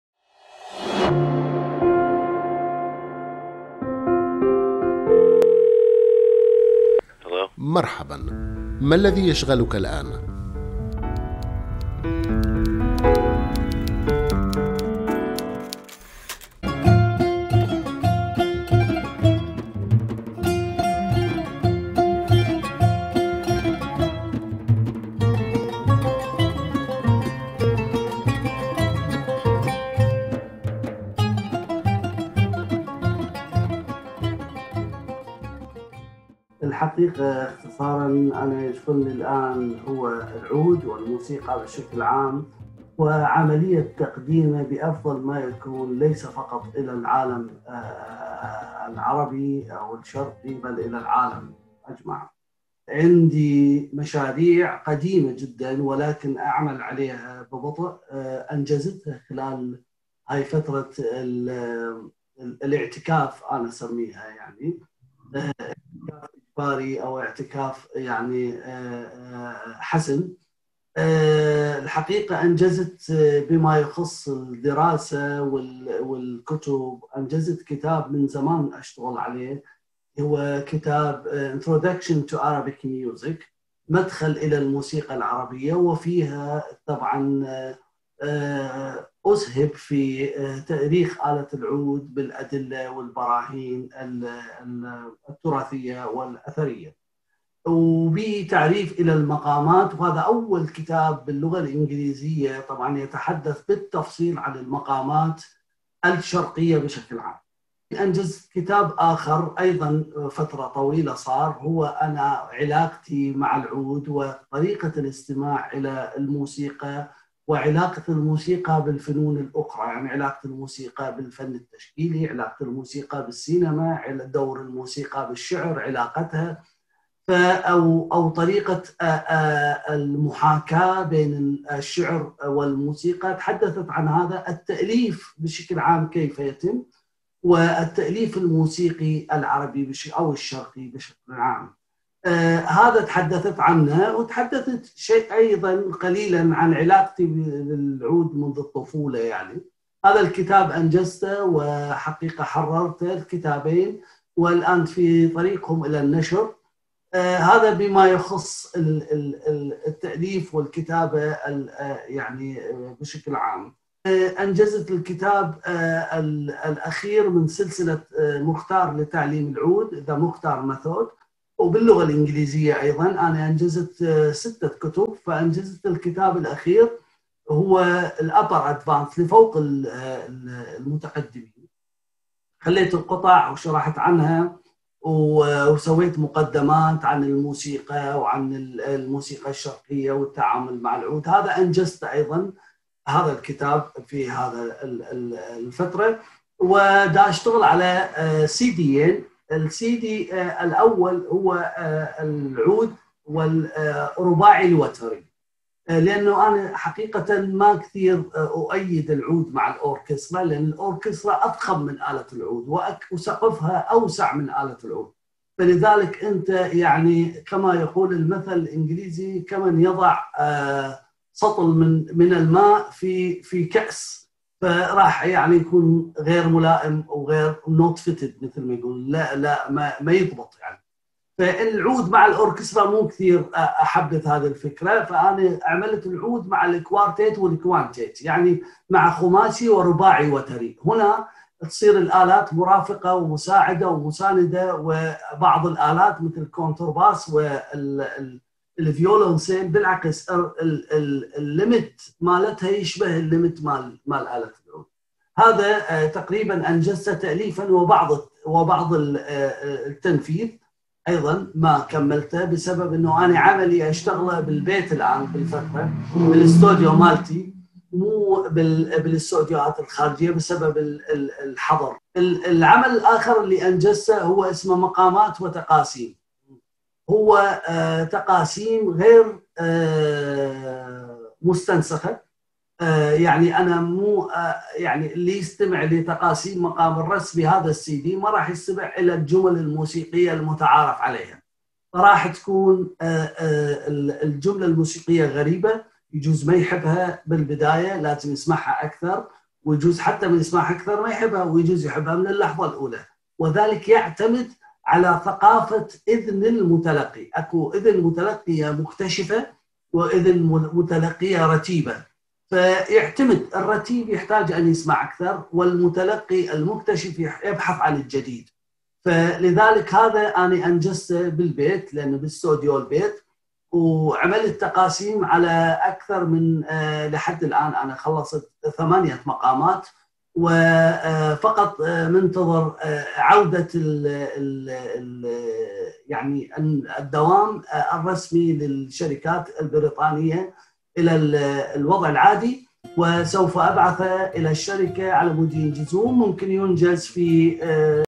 إنشغالات الموسيقار العراقي أحمد مُختار الراهنة
أجرى الحوار: عرفان رشيد
أحمد مختار: " حوّلت فترة الحجر الإجبارية إلى مرحلة اعتكافٍ مُثمرة أعدت فيها صياغة أشياء كيرة وأكملت ما كنت قد تركت جانباً بسبب الانشغالات.. كتابةً وتأليفاً..".